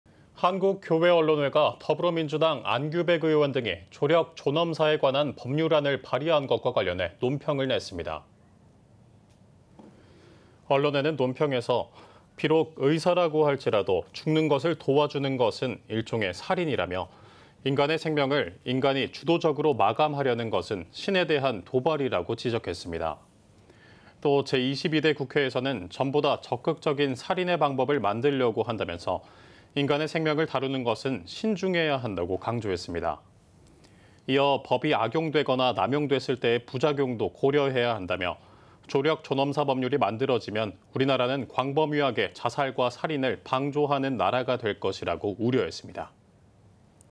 한국교회언론회, ‘조력존엄사에 관한 법률안’ 발의에 “신에 대한 도발” (이한승)ㅣCTS뉴스
한국교회언론회가 더불어민주당 안규백 의원 등이 ‘조력존엄사에 관한 법률안’을 발의한 것과 관련해 논평을 냈습니다.

언론회는 논평에서 “비록 의사라고 할지라도 죽는 것을 도와주는 것은 일종의 살인”이라며 “인간의 생명을 인간이 주도적으로 마감하려는 것은 신에 대한 도발”이라고 지적했습니다.

또 “제22대 국회에서는 전보다 적극적인 살인의 방법을 만들려고 한다”면서 “인간의 생명을 다루는 것은 신중해야 한다”고 강조했습니다.

이어 “법이 악용되거나 남용됐을 때의 부작용도 고려해야 한다”며 “조력존엄사 법률이 만들어지면 우리나라는 광범위하게 자살과 살인을 방조하는 나라가 될 것”이라고 우려했습니다.